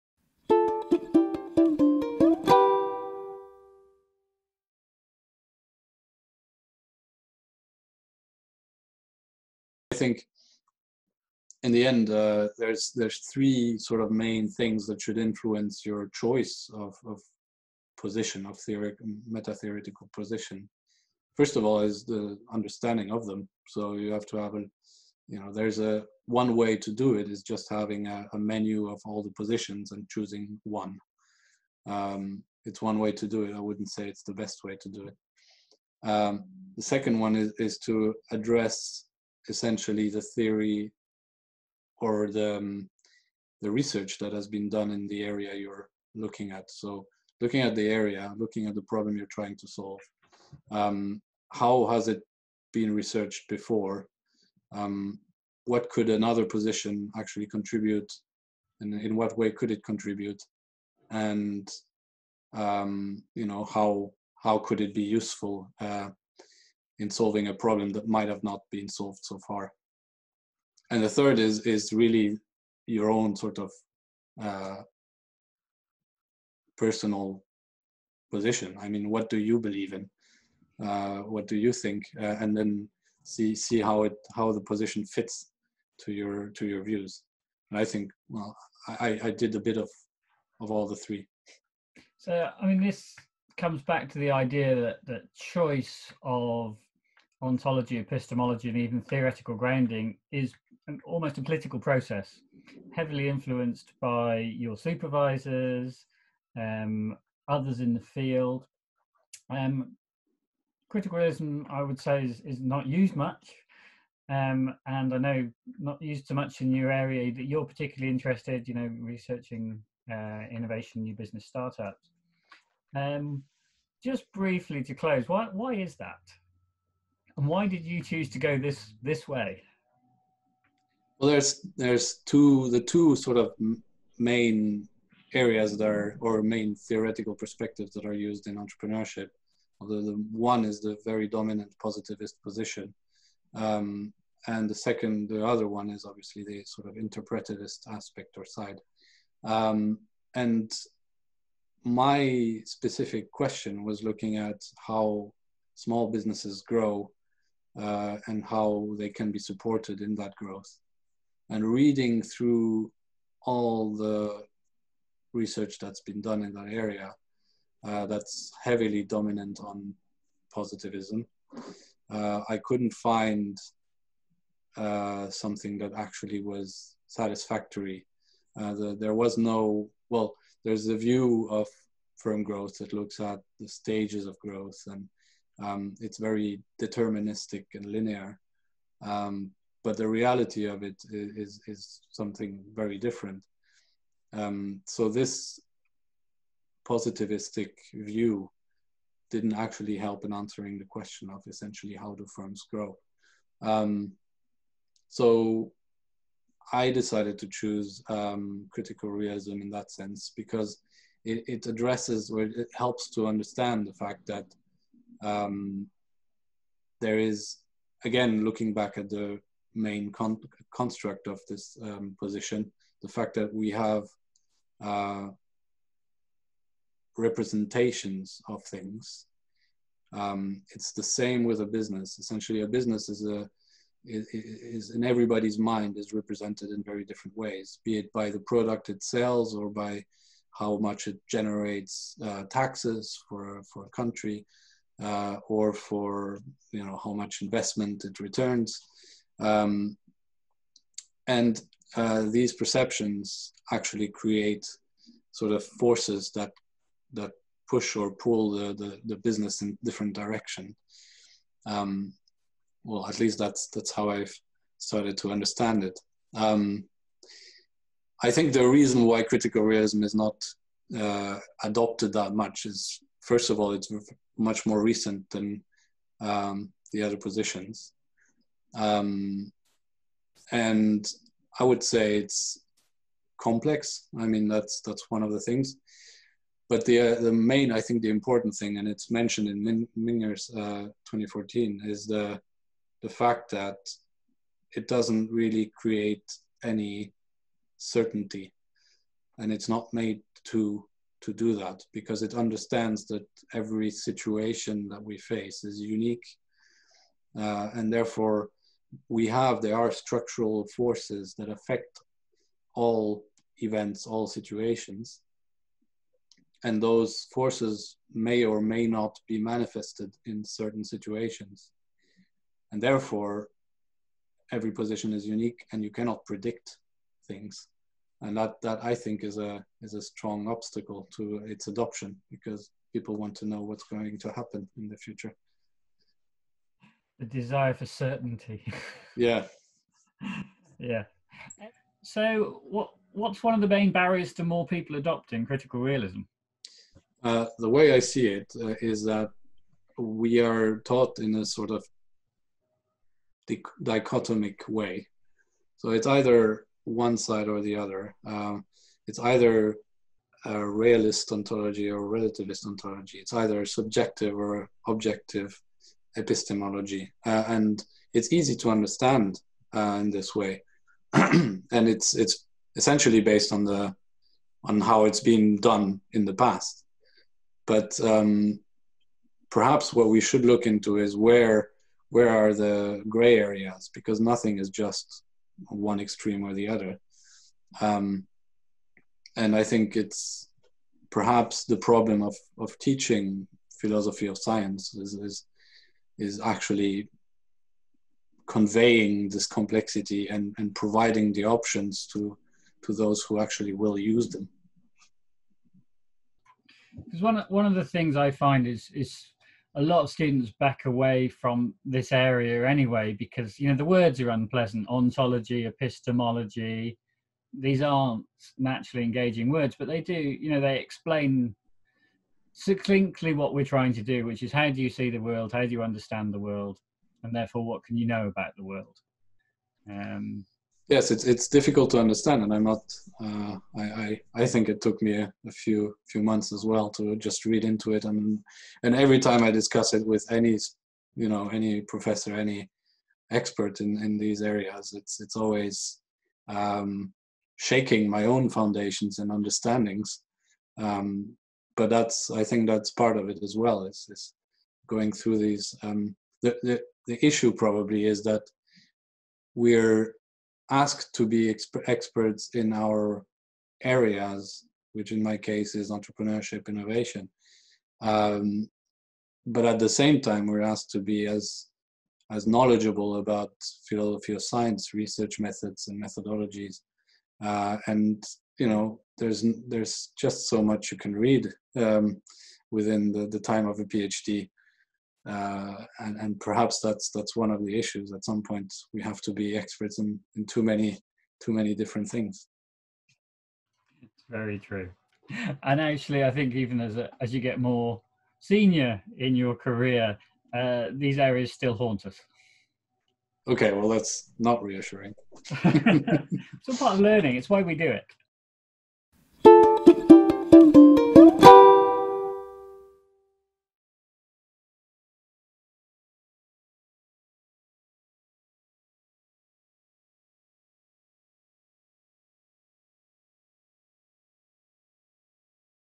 Critical Realism - why choose Critical Realism? Epistemology selection
Critical Realism is a relatively new epistemological position. Choosing which ontology, epistemology and methods you might adopt is always difficult. It is largely a political decision, influence by those around you. Here, we discuss this choice and propose the CR may be an interesting option that offers new insights into an area.

You may be interested in other videos on this channel.